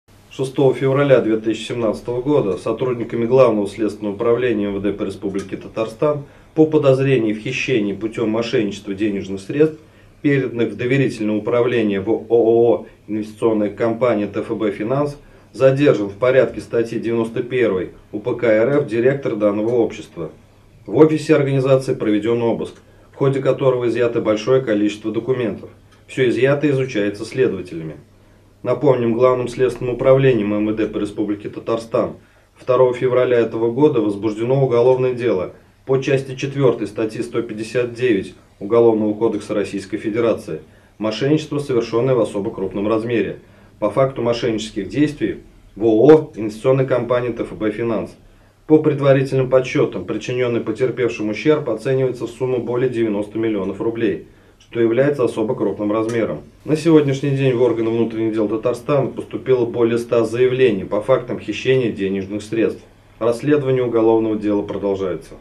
МВД прокомментировало задержание директора «ТФБ Финанс»
В офисе организации проведен обыск, изъято большое количество документов